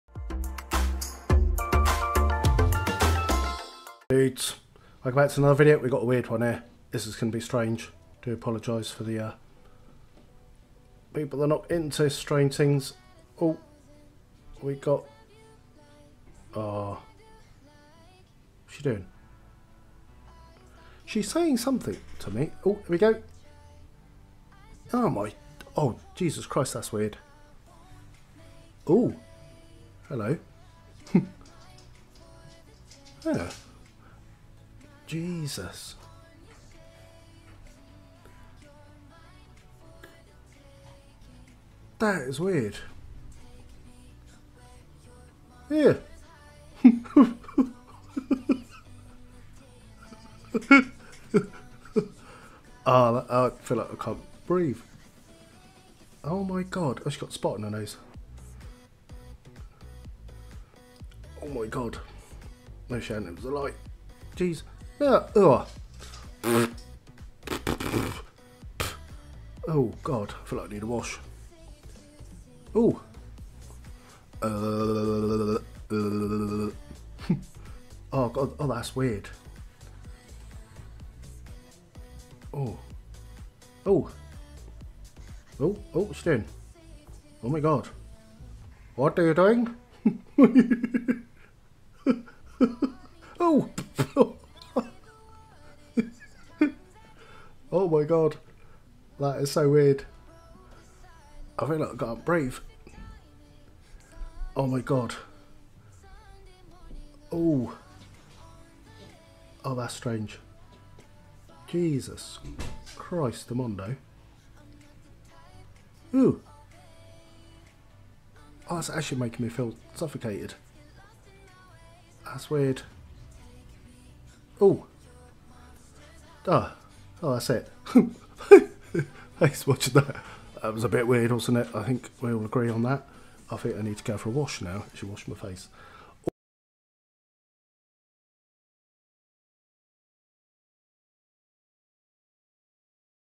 VR Girls of the Internet - I Need a Wash Now lol - Meta Quest 3
Hi
Welcome to my vid.
My plan is to search the depths of the internet and find as many VR vids of Females as i can and see what they are up to so you don't have to lol.
Finding some really interesting stuff and some really strange stuff and just what the hell was that i just watched.
If you do not have a VR headset its hard to explain what it is actually like to watch these vids so go get your self one and you will find out.
Also you can go Hardcore and Watch VR Porn but cant show that on YouTube as i will get told off lol.

Thanks

Phil